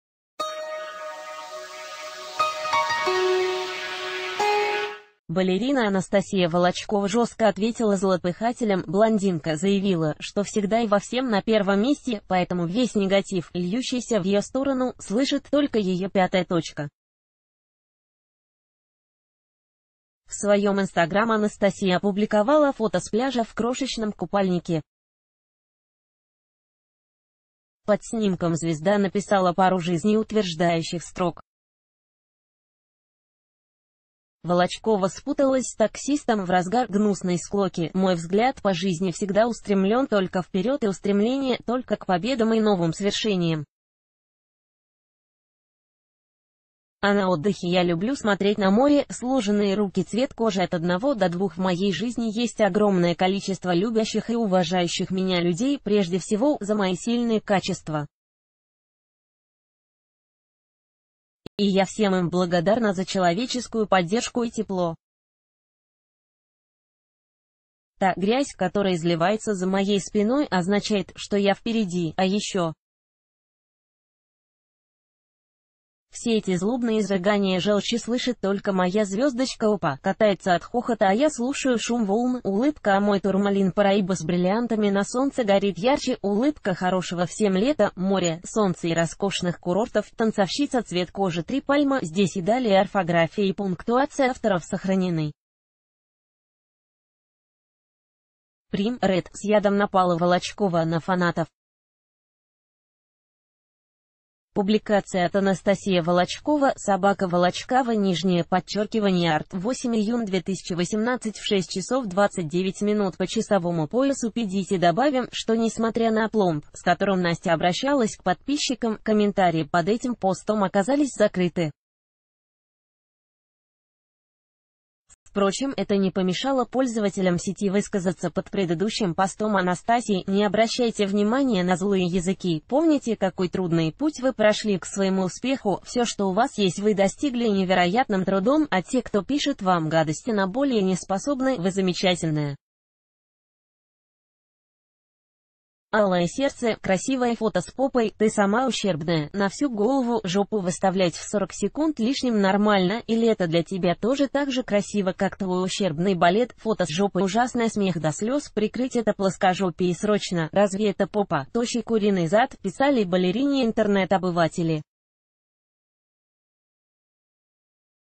Волочкова с "куриным задом" совершила дерзкое нападение | TVRu
Балерина Анастасия Волочкова жестко ответила злопыхателям. Блондинка заявила, что всегда и во всем на первом месте, поэтому весь негатив, льющийся в ее сторону, "слышит" только ее пятая точка. В своем Instagram Анастасия опубликовала фото с пляжа в крошечном купальнике. Под снимком звезда написала пару жизнеутверждающих строк. Волочкова спуталась с таксистом в разгар гнусной склоки "Мой взгляд по ...